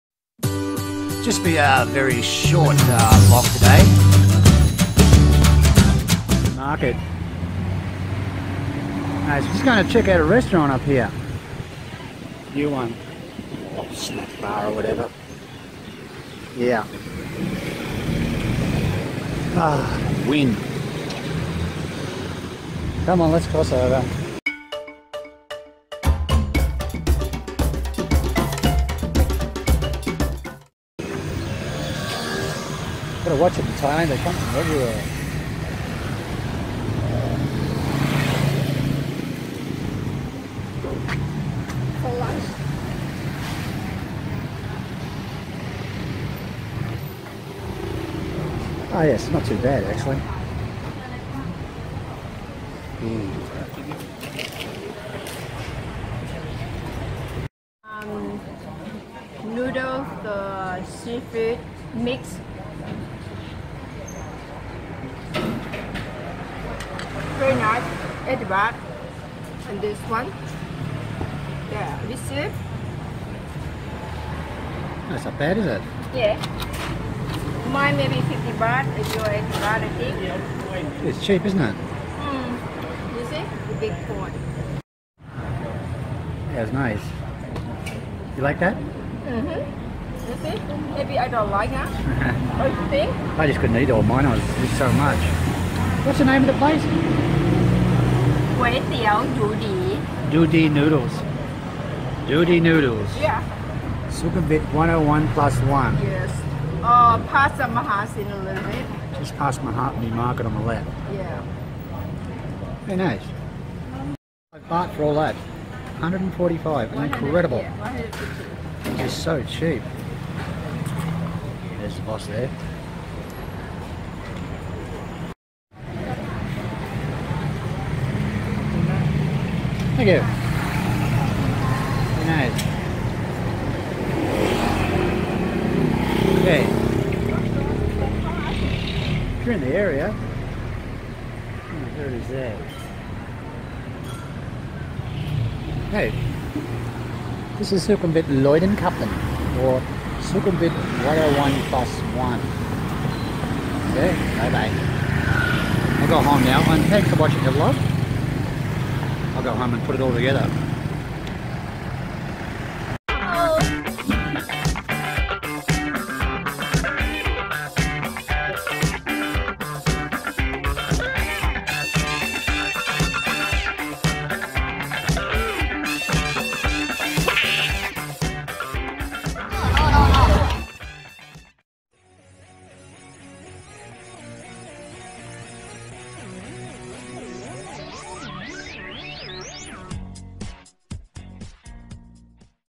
DU DI Noodles Shop Bangkok Thailand, Vlog 057
Thailand Unplugged8 hours ago
No Videos of Thailand will be uploaded until Saturday the 22nd October. Out of Respect for the King

Today just a short one we take you to a Noodle shop that is very cheap and very very good